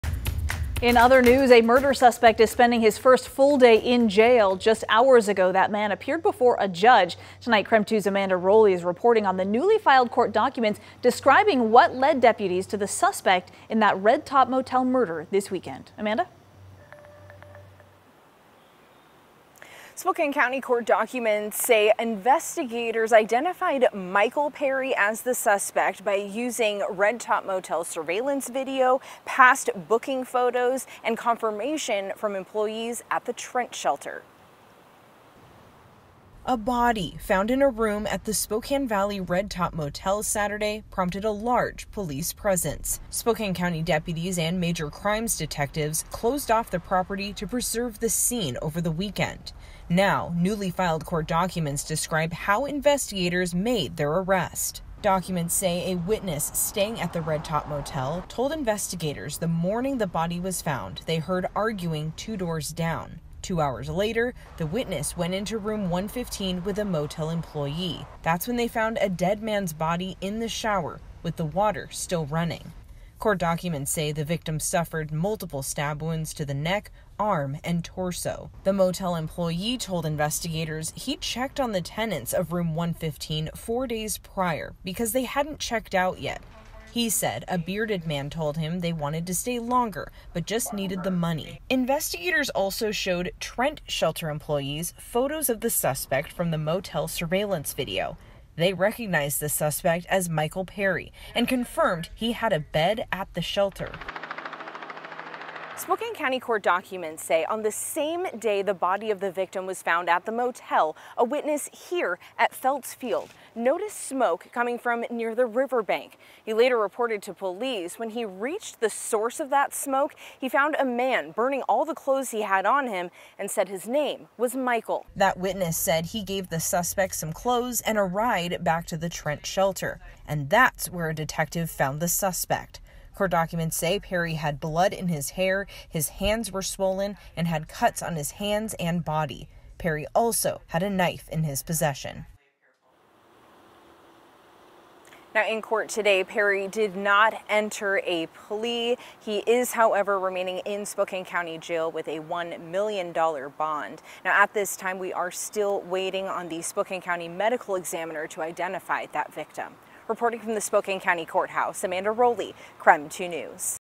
Suspect in Red Top Motel murder appears in court
KREM 2 is still waiting on the Spokane County Medical Examiner to identify the victim in the murder.